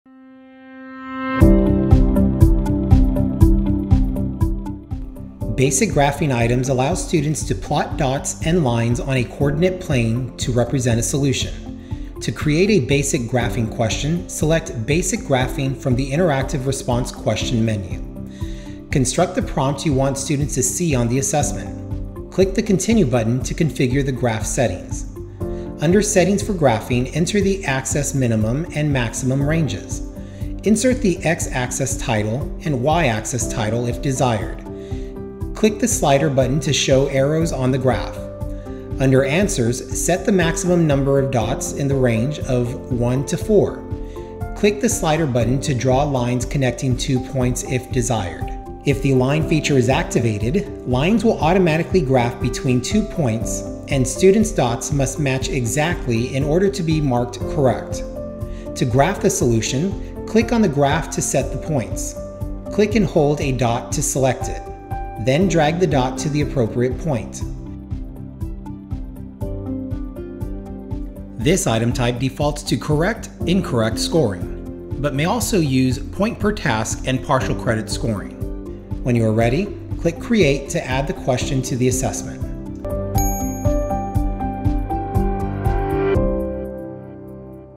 Interactive Item Types: Create a Basic Graphing Item
Basic graphing items are available for online assessments in Aware.